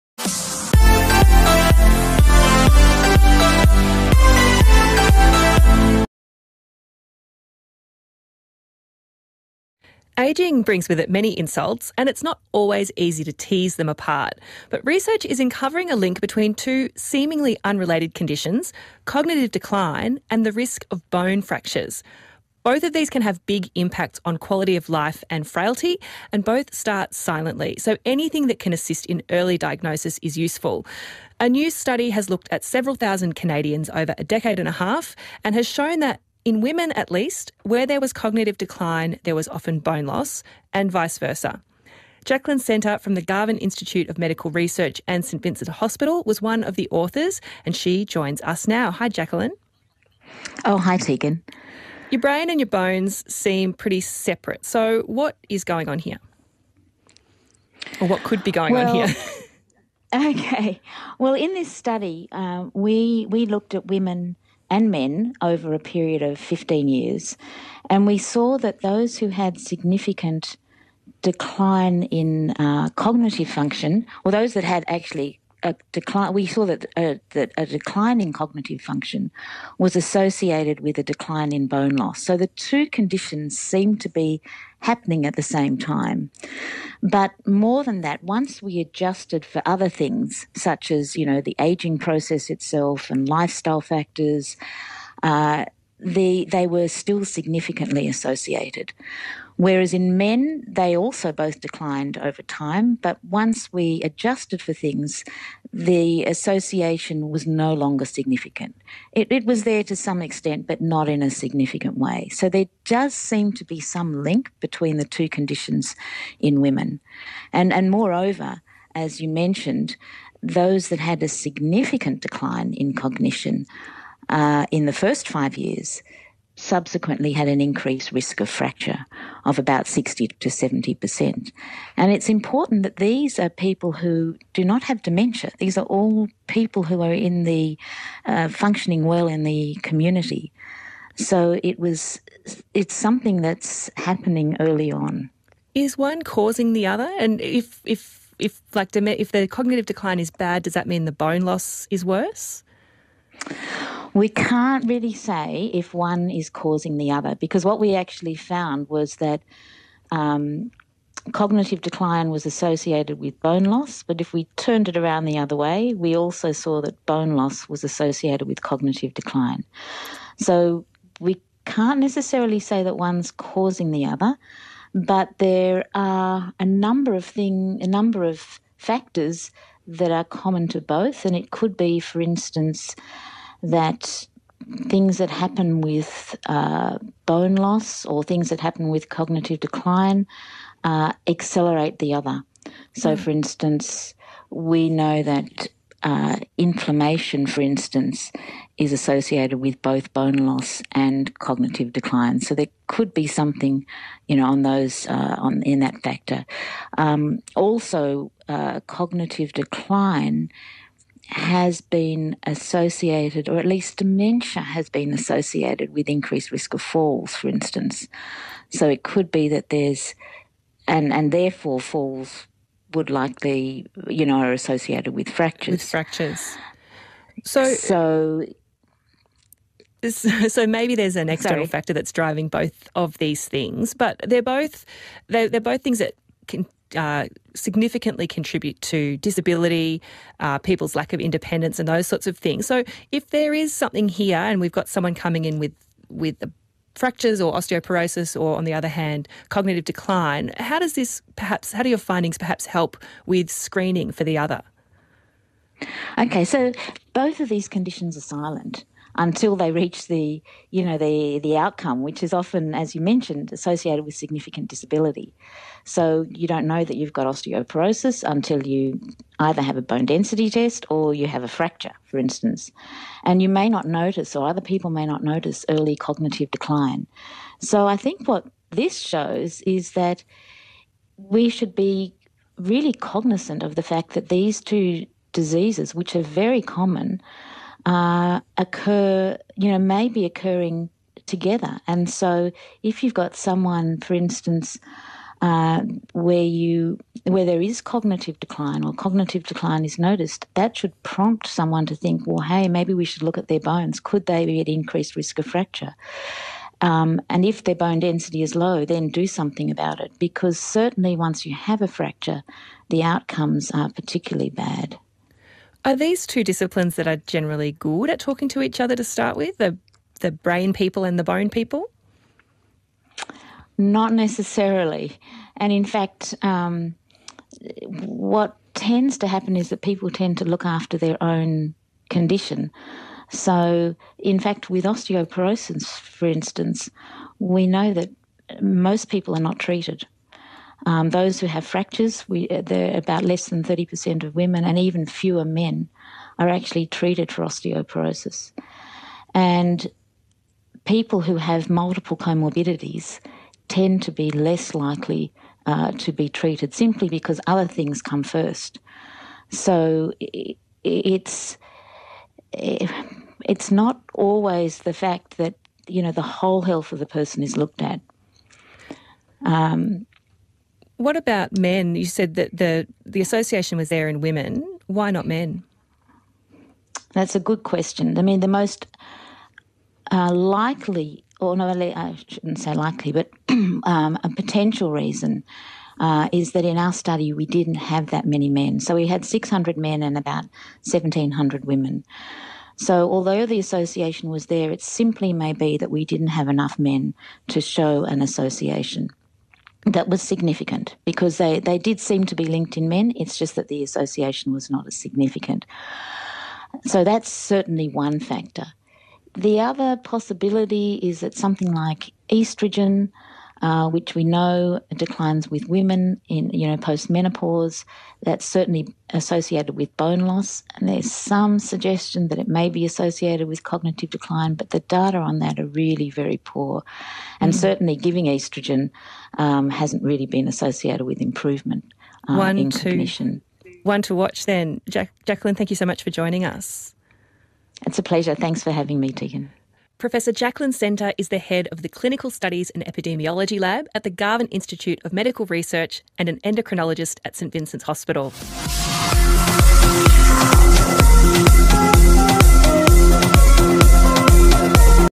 OET LISTENING PODCAST 14. FRACTURES IN ELDERLY
The link between cognitive decline and fracture